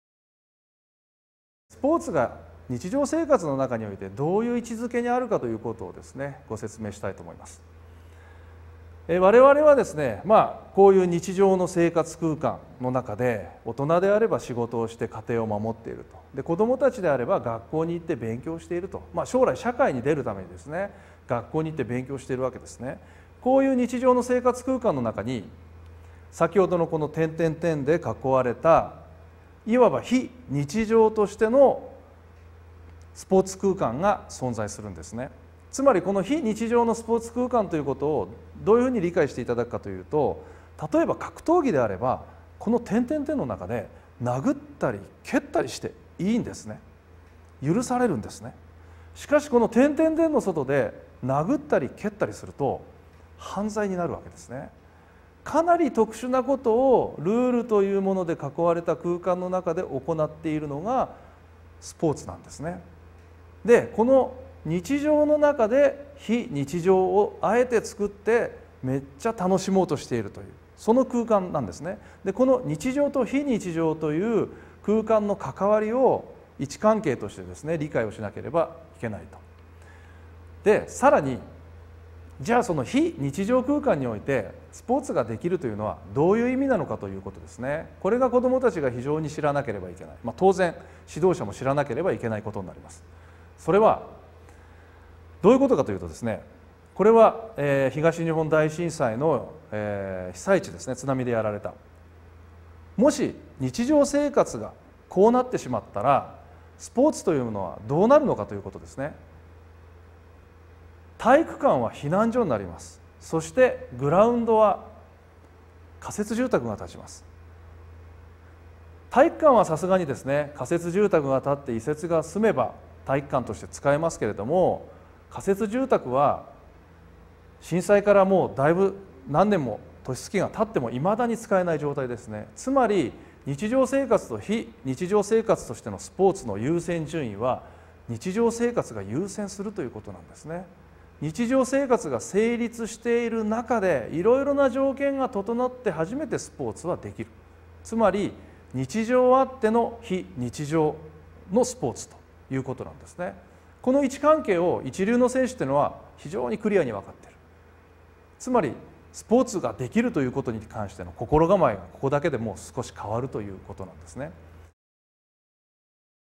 Disc1スポーツの位置を知る
ジャパンライムDVD 【ボイス】
『一流の「スポーツマンのこころ」選手がこれを理解すれば頑張れる』

■指導／解説：髙橋　正紀（岐阜経済大学経営学部教授・医学博士　岐阜経済大学サッカー部監督　 JFA　A級コーチ・GFAチーフインストラクター）
■実技協力：西濃地区トレーニングセンター選手